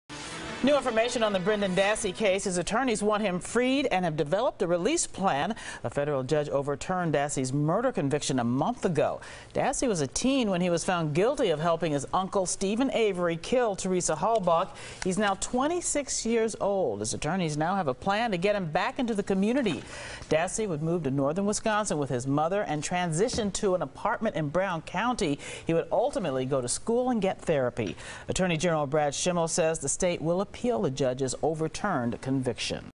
Brendan Dassey Lawyers Seek Release
Lawyers want Dassey to be released on bond.